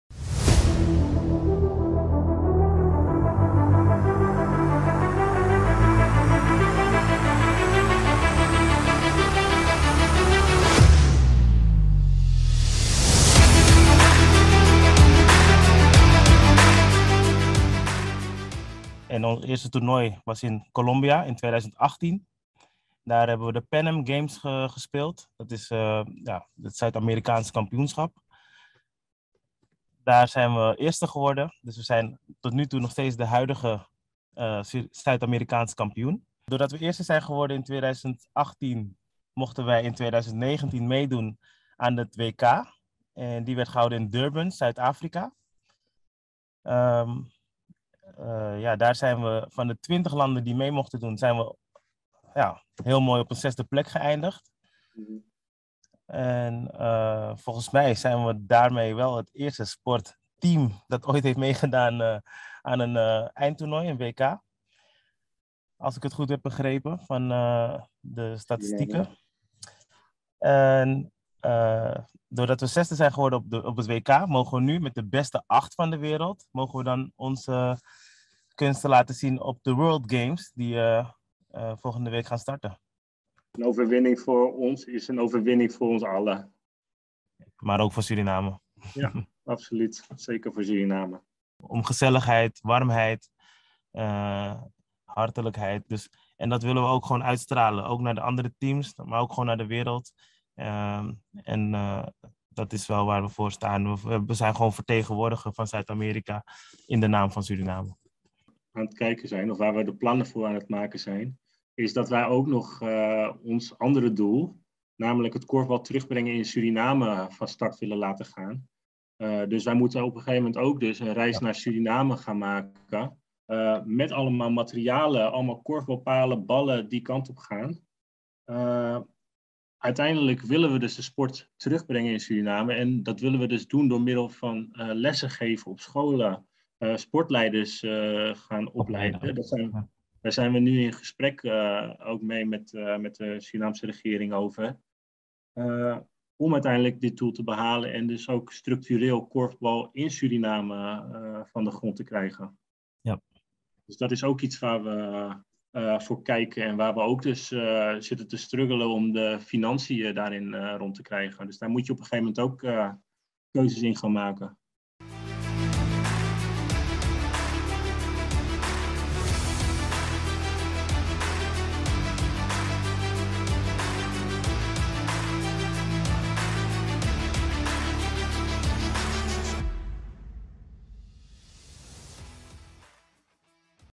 SURINAAMS KORFBALTEAM REIST AF NAAR WORLD GAMES
Op zondag 10 juli 2022 vertrekt het Surinaamse korfbalteam vanuit Nederland naar Birmingham, Alabama, in de Verenigde Staten van Amerika om Suriname te vertegenwoordigen op de World Games. Het betreft een internationaal sportevenement voor niet-olympische sporten dat om de vier jaar wordt georganiseerd. Het korfbalteam, dat op 2 juli jongstleden zijn laatste oefenwedstrijd had, zal van 13 tot en met 17 juli 2022 aan de World Games deelnemen.